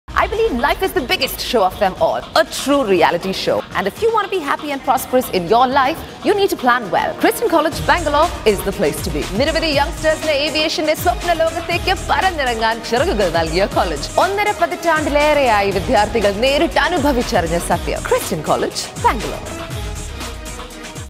CHRISTIAN COLLEGE BANGALORE
Our Christian College, Bangalore, India was founded in the year 2000, is run under the aegis of ‘Sri Rajaram Mohan Roy Educational Trust by the congregation of experts in diverse fields to advocate a gallant root for providing eminent education. The beginning of different educational programs under the Christian Group was with the sole aim of offering quality education to youth of all sections of society.

BSc Optometry
BBA Aviation
BCA Cloud Computing
BSc Cardiac Care Technology
BSc Perfusion Technology
BSc Medical Imaging Technology